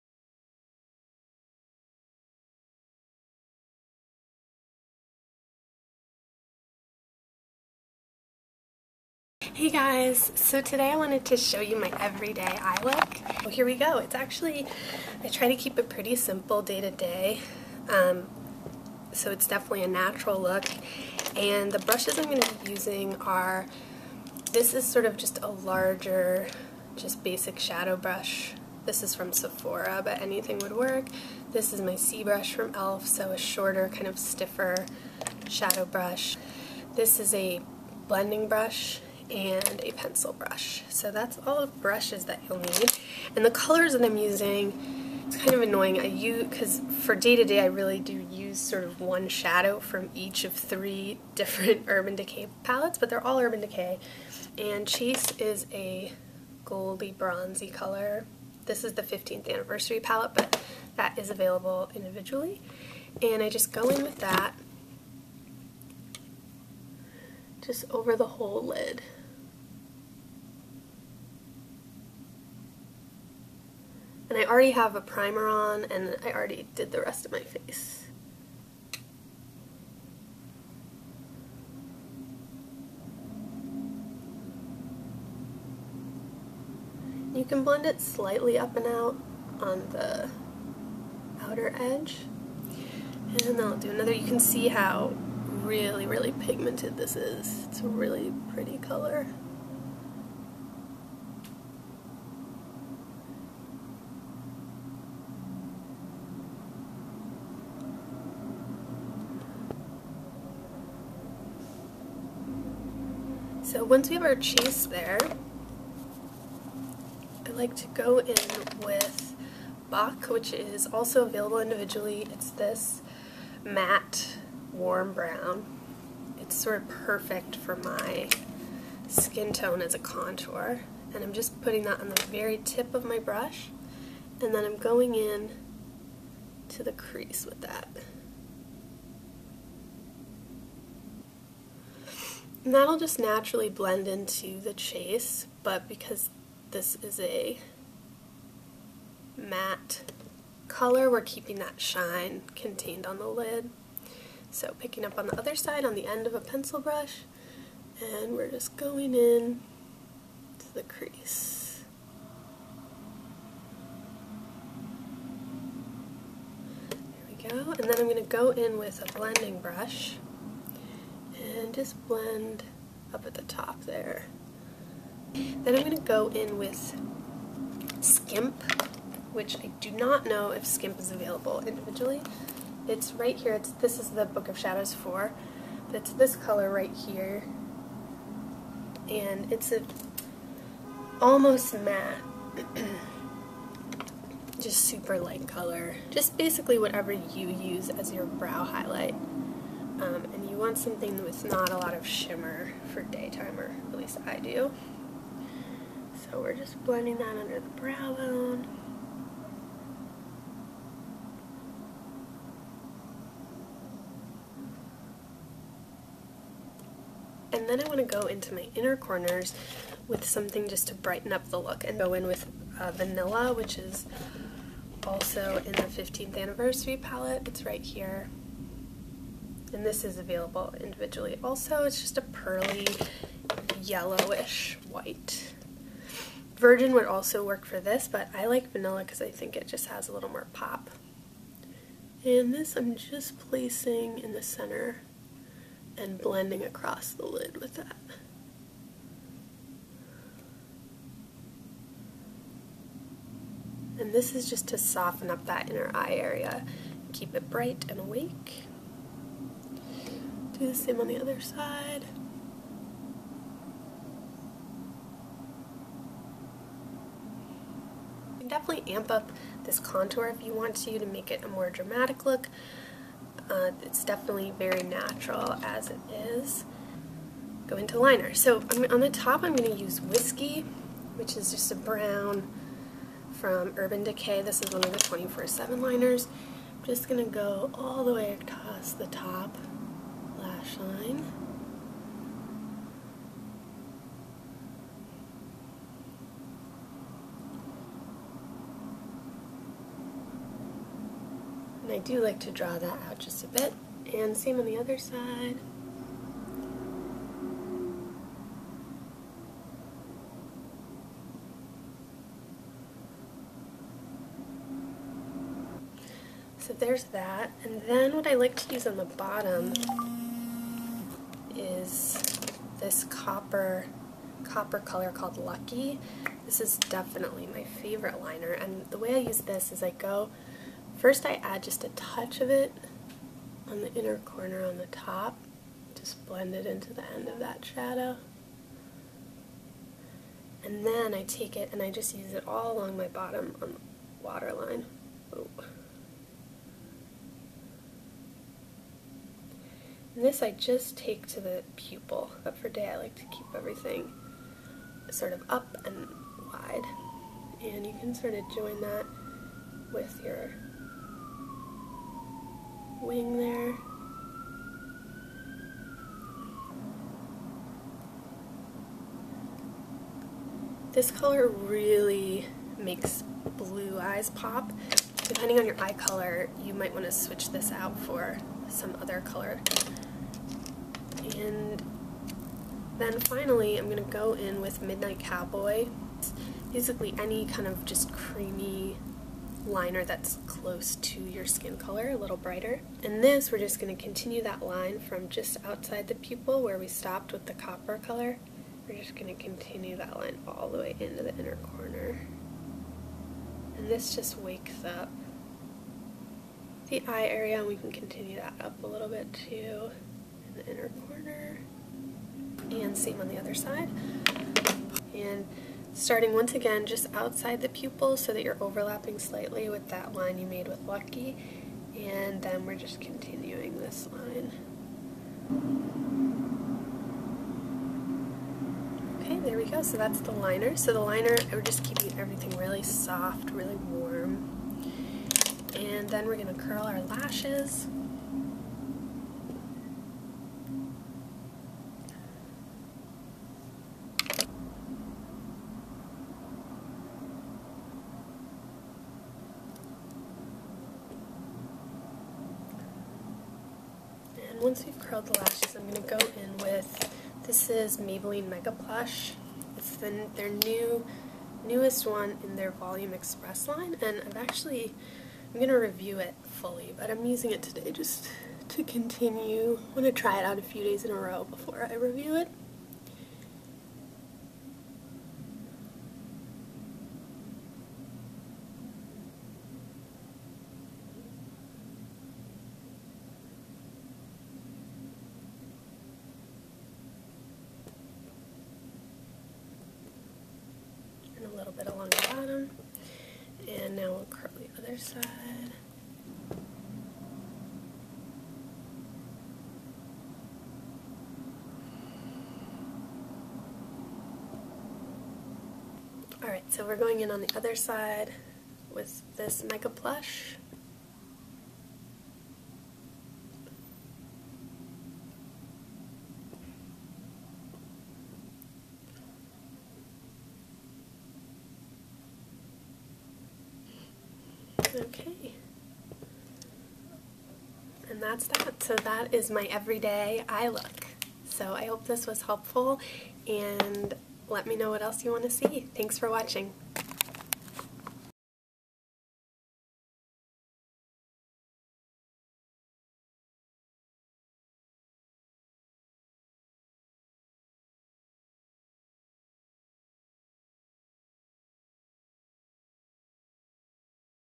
my everyday neutral eye look
This is my quick and easy everyday eye look. I use Urban Decay in this video- but any neutral shadows would work! Blue-eyed girls, this will really bring out your eye color!